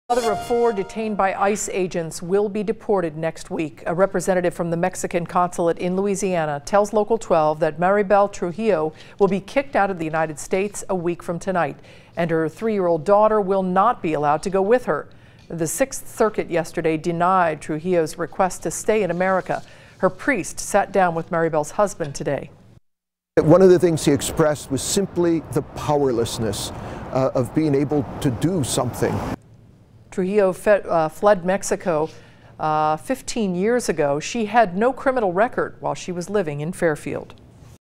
Local mother to be deported on April 19
CINCINNATI (WKRC) - More information has been revealed in the continuing story of a local mother of four from Fairfield who is being deported to Mexico.

According to the Consul of Mexico from Louisiana, Maribel Trujillo is scheduled for removal on Wednesday, April 19. The plans to remove her do not include any of her children.

"Mrs. Maribel Trujillo life has been turned upside down. She is scared for her future, but mostly scared for her childrens’ future. She is the sole provider for her family and they all depend on her. The uncertainty and sense of helplessness is palpable," said Carlos Ponce, who visited Maribel at the LaSalle Detention Facility.

"The Mexican government is in contact with Ms. Trujillo and her legal representative. It will carry out the necessary actions to ensure her rights are respected, including making sure her removal is conducted in an orderly and safe manner, after having exhausted all available legal remedies," said Ponce.

Larry Davis reached out to the Consul of Mexico in New Orleans, Lousiana regarding Maribel Trujillo. The following is their response in full:

Good Afternoon Mr. Davis,
Thank you for your interest in this case, I’m sending you my comments regarding this case.
I’m coming from LaSalle Detention Facility, I visited her and as you can imagine, Mrs. Maribel Trujillo life has been turned upside down. She is scared for her future, but mostly scared for her childrens’ future. She is the sole provider for her family and they all depend on her. The uncertainty and sense of helplessness is palpable.
Her removal is scheduled for Wednesday, April 19, 2017. from Louisiana do not involve minors. If Ms. Trujillo is unable to return with her daughter, a member of the consulate can accompany Daniella to Mexico once her mother is there and ready to receive her. For the safety of the detainees, I can only say she will be removed to the interior of Mexico, but cannot say where.
In adition, The Consulate of Mexico in New Orleans are addressing the detention and removal process of Ms. Maribel Trujillo, a Mexican citizen and mother of 4 children, all of whom are U.S. citizens.
Ms. Trujillo was detained at her home in Fairfield, Ohio, with the purpose of proceeding with an order of removal. There is no information indicating that she is a risk to the national security of the United States, nor of her having any criminal history in this country. Ms. Trujillo is the sole provider for her four children, two of which have special needs. She has worked and contributed greatly to her community in the United States since 2002.
The Mexican government is in contact with Ms. Trujillo and her legal representative. It will carry out the necessary actions to ensure her rights are respected, including making sure her removal is conducted in an orderly and safe manner, after having exhausted all available legal remedies.
The Consulate of Mexico reaffirms its commitment to providing consular protection and legal aid to all Mexican people, no matter their immigration status.
-Carlos Ponce, Consul of Mexico